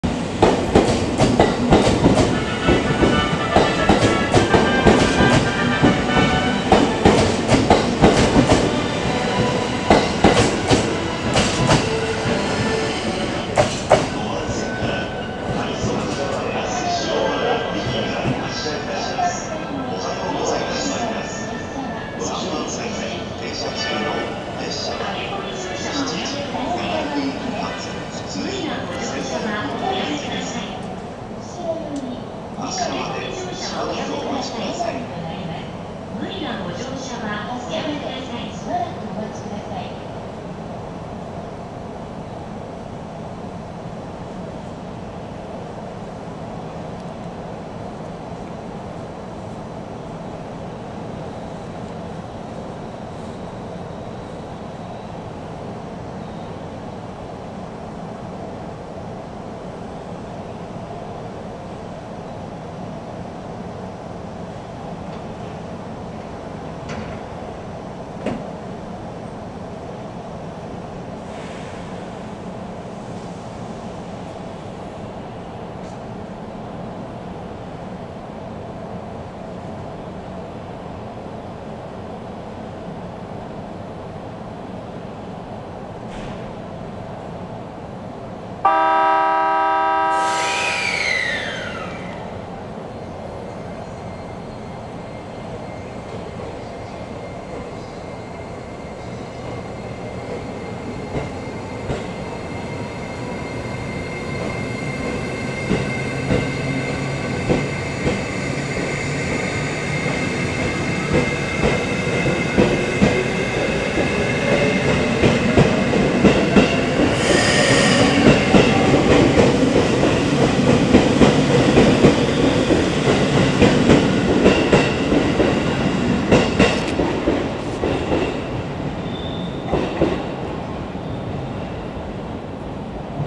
E653系1000番台(U101編成) 臨時快速那須塩原行 仙台駅 発車シーン
2/21～23 運行した臨時快速那須塩原行
仙台駅の発車シーンをどうぞ。